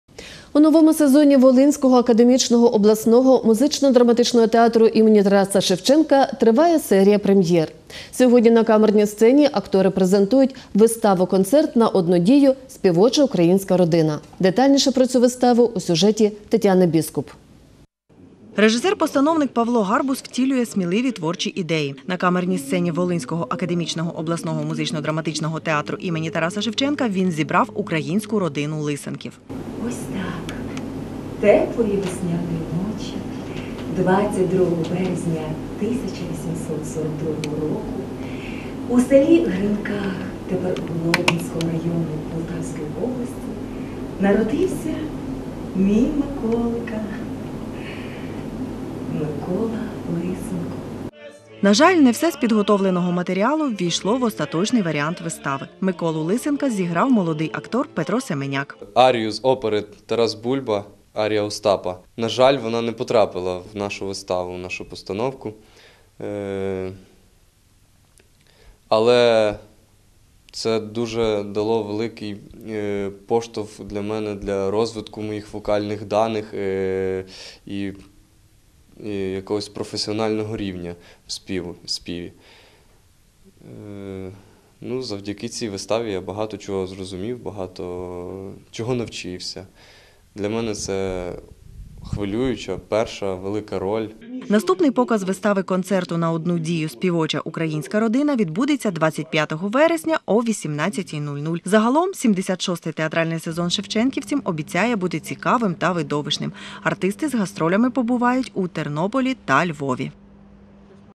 На сцені Волинського облмуздрамтеатру ще одна прем’єра
У новому сезоні Волинського академічного обласного музично-драматичного театру імені Тараса Шевченка триває серія прем’єр. Зокрема, на камерній сцені актори презентували виставу-концерт на одну дію «Співоча українська родина». Наступний показ вистави – концерту на одну дію «Співоча українська родина» відбудеться 25 вересня о 18.00. Загалом, 76 театральний сезон шевченківців обіцяє бути цікавим та видовищним. Артисти з гастролями побувають у Тернополі та Львові.